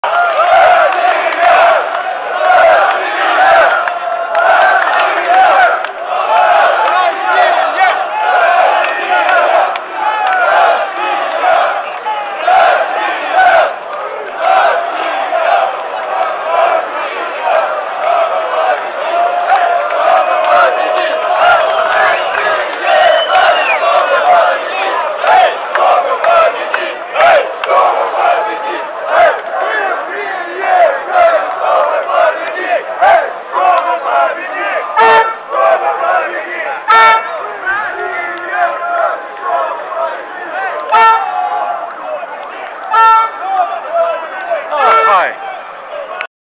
Marsz Rosjan. Byli i normalni polscy kibice...
Marsz rosyjskich kibiców na Stadion Narodowy przerwany przez Polaków skandujących 'ruska kurwa, a eja eja o' i 'jebać Rosję'. Na szczęście byli też i normalni polscy kibice, którzy skanowali 'Polska, biało-czerwoni', na co Rosjanie odpowiadali: 'Rasija, Rasija!' i 'My tu prijechali, sztoby pobiedit'...